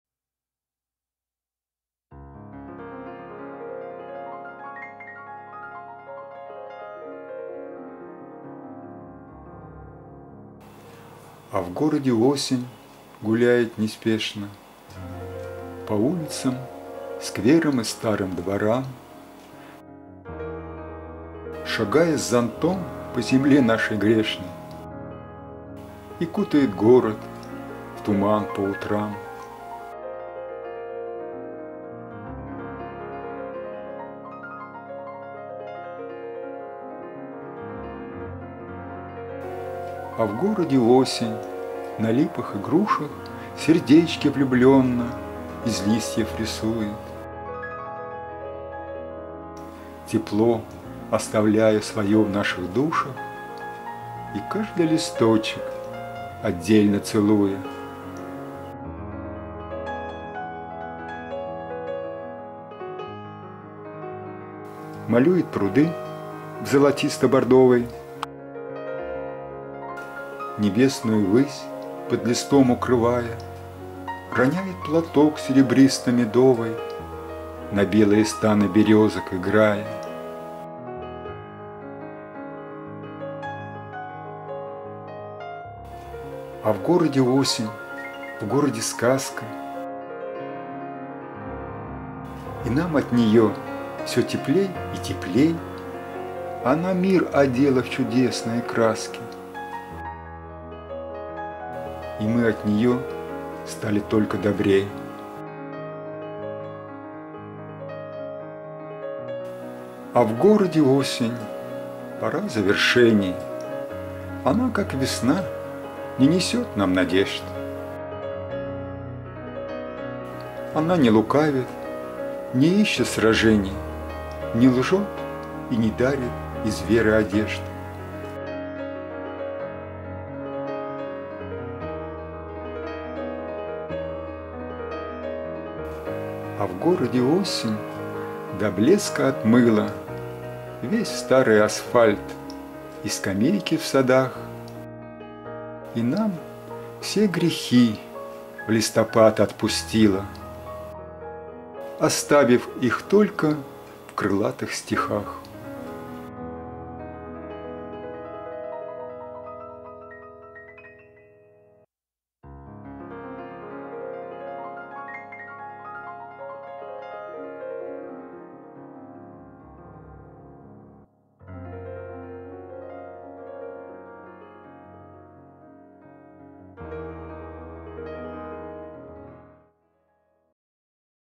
🍁А в городе осень гуляет неспешно
А в городе осень
Анна Копылова
По улицам, скверам и старым дворам,
Шагая с зонтом по земле нашей грешной,
И кутает город в туман по утрам.

А в городе осень на липах и грушах
Сердечки влюблённо из листьев рисует,
Тепло оставляя своё в наших душах,
И каждый листочек отдельно целуя.

Малюет пруды в золотисто - бордовый,
Небесную высь под листом укрывая,
Роняет платок  серебристо -  медовый
На белые станы берёзок, играя.

А в городе осень, в городе сказка
И нам от неё всё теплей и теплей.
Она мир одела в чудесные краски
И мы от неё стали только добрей.

А в городе осень, пора завершений,
Она, как весна, не несёт нам надежд.
Она не лукавит, не ищет сражений,
Не лжёт и не дарит из веры одежд.

А в городе осень до блеска отмыла
Весь старый асфальт и скамейки в садах.
И нам все грехи в листопад отпустила,
Оставив их только в крылатых стихах.